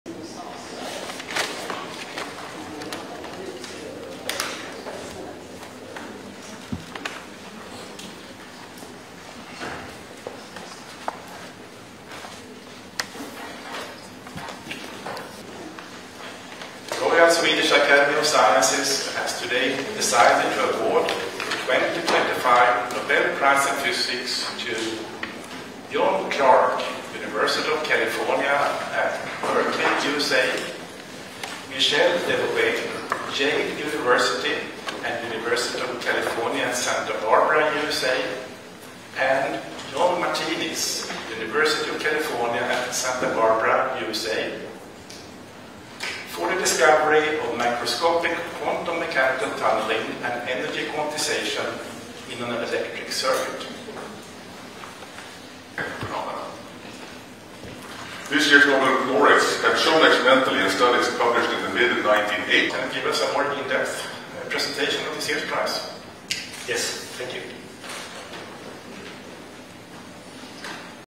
Clarke, Devoret and Martinis win the Nobel Prize in Physics
RESTRICTION SUMMARY:

ASSOCIATED PRESS
Stockholm, Sweden - 7 October 2025
++AUDIO AS INCOMING++
1. Panel entering
2, SOUNDBITE (English) Hans Ellegren, Secretary-General, Royal Swedish Academy of Sciences:
"The Royal Swedish Academy of Sciences has today decided to award the 2025 Nobel Prize of Physics to John Clarke, University of California at Berkeley, USA, Michel, H Devoret, Yale University and University of California at Santa Barbara, USA and John Martinis, University of California at Santa Barbara USA, for the discovery of microscopic quantum mechanical tunnelling and energy quantization in an electric circuit."
3. Various of laureates on screen
STORYLINE:
John Clarke, Michel H. Devoret and John M. Martinis won the Nobel Prize in Physics on Tuesday for research into quantum mechanical tunneling.

Clarke conducted his research at the University of California, Berkeley; Martinis at the University of California, Santa Barbara; and Devoret at Yale and also at the University of California, Santa Barbara.

The Nobel committee said that the laureates' work provides opportunities to develop “the next generation of quantum technology, including quantum cryptography, quantum computers, and quantum sensors.”

It is the 119th time the prize has been awarded.

Last year, artificial intelligence pioneers John Hopfield and Geoffrey Hinton won the physics prize for helping create the building blocks of machine learning.

Nobel announcements continue with the chemistry prize on Wednesday and literature on Thursday. The Nobel Peace Prize will be announced Friday and the Nobel Memorial Prize in economics on Oct. 13.

The award ceremony will be held Dec. 10, the anniversary of the 1896 death of Alfred Nobel, the wealthy Swedish industrialist and the inventor of dynamite who founded the prizes.

The prizes carry priceless prestige and a cash award of 11 million Swedish kronor (nearly $1.2 million).